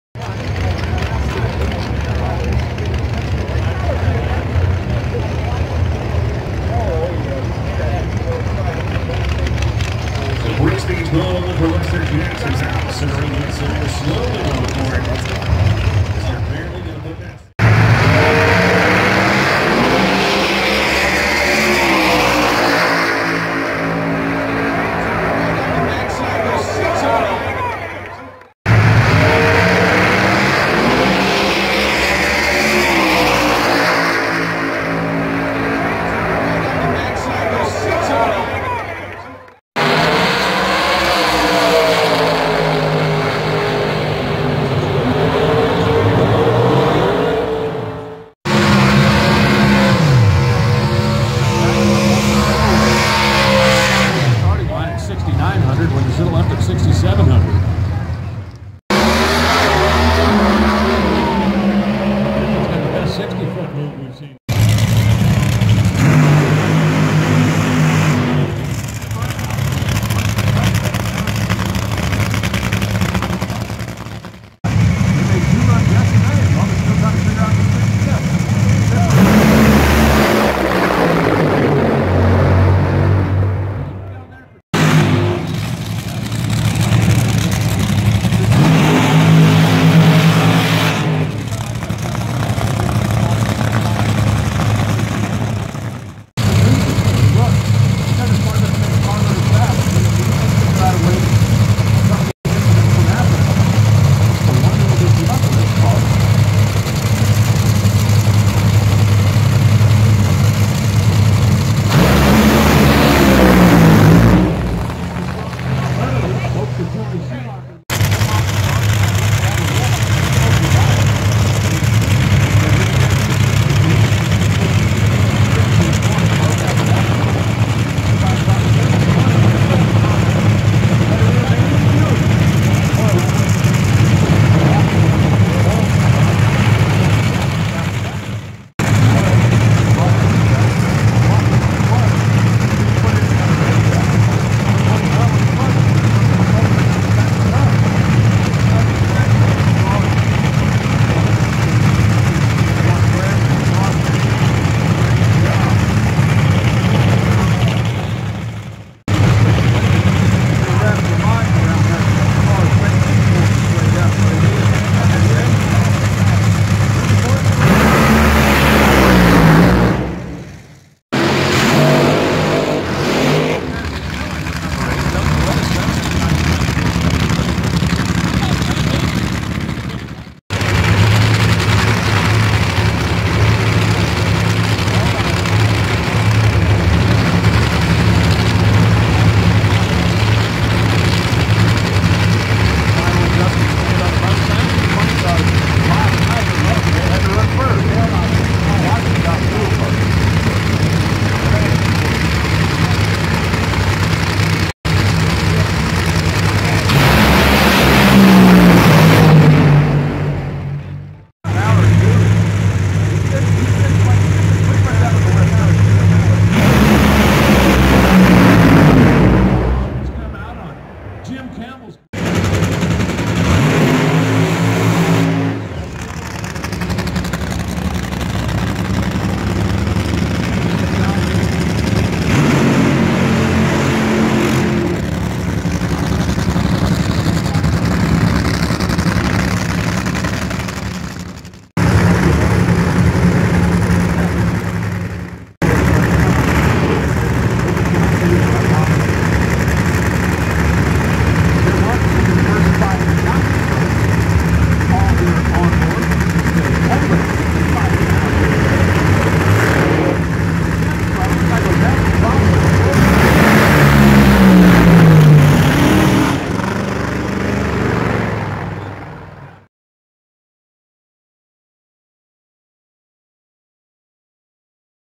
2018 NHRA 49th Gatornationals Highlights
2018 NHRA 49th Gatornationals Highlights. Pro mods, Sportsman Funny Car, Top Fuel